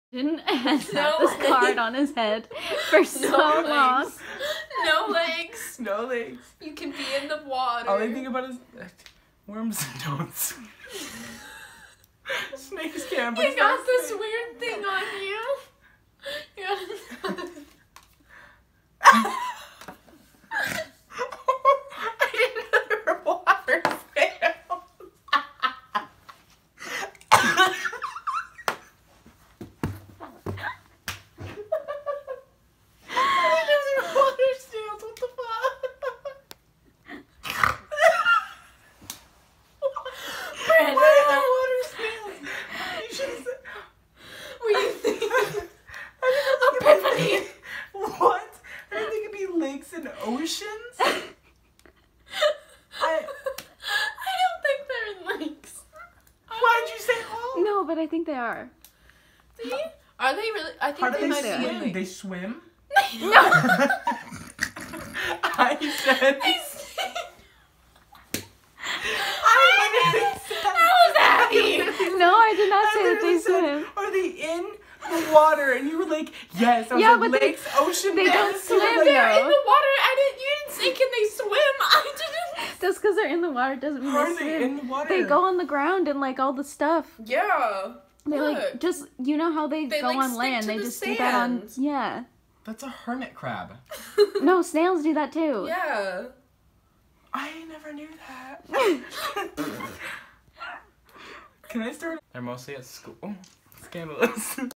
brandon finds out about snails playing headbands
this is a really short video that shows brandon’s true character. he had no idea water snails exist. this is him finding out about them.